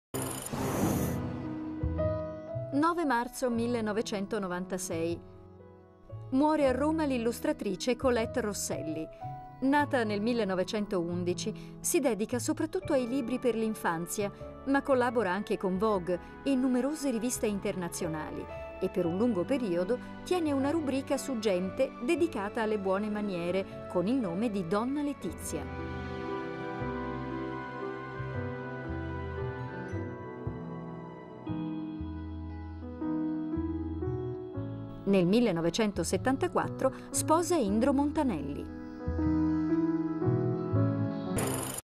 (anniversari morte 1996) 09 marzo, Roma: Colette Rosselli illustratrice, sposa di Montanelli
Colette Cacciapuoti Rosselli, nota anche con lo pseudonimo di Donna Letizia (Losanna, 25 maggio 1911 – Roma, 9 marzo 1996), è stata una scrittrice, illustratrice e pittrice italiana. È nota soprattutto per la seguita rubrica di bon ton "Il saper vivere", che ha tenuto prima su Grazia e in seguito su Gente.
Come illustrarice aveva iniziato a pubblicare i suoi disegni a New York per le riviste Harper's Bazaar, Mademoiselle e Vogue, illustrando anche libri per bambini, alcuni dei quali scritti da lei stessa.
 == Illustrazioni ==
* Libri di altri autori illustrati da Colette Rosselli.
* Angiolo Silvio Novaro Il cestello, Arnoldo Mondadori Editore, 1951. ISBN non esistente
* Franca Valeri Il diario della signorina snob, Arnoldo Mondadori Editore, 1951. ISBN non esistente
* Wilhelm Hauff Naso Nasone e altri racconti, Arnoldo Mondadori Editore, 1951. ISBN non esistente
* Fratelli Grimm Pollicino in viaggio e altre fiabe, Arnoldo Mondadori Editore, 1952. ISBN non esistente
* Hans Christian Andersen Le scarpette rosse e altri racconti, Arnoldo Mondadori Editore, 1953. ISBN non esistente
* John Ruskin Il re del fiume d'oro, Arnoldo Mondadori Editore, 1953. ISBN non esistente
* Indro Montanelli Storia di Roma, Longanesi, 1957. ISBN non esistente
* William Cole I went to the animal fair, The World Publishing Company, 1958. ISBN non esistente
* Indro Montanelli Storia dei greci, Rizzoli, 1959. ISBN non esistente
* Alvar Gonzáles Palacio, Giorgio Soavi Disegni di Colette Rosselli, Longanesi, 1983.
* Franca Valeri Il diario della signorina snob, Edizioni Lindau, 2003. ISBN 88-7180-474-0 (ristampa

Arlunydd benywaidd o'r Eidal oedd Colette Rosselli (25 Mai 1911 - 9 Mawrth 1996).
Fe'i ganed yn Lausanne a threuliodd y rhan fwyaf o'i hoes yn ddinesydd o'r Eidal. Bu'n briod i Indro Montanelli. Bu farw yn Rhufain.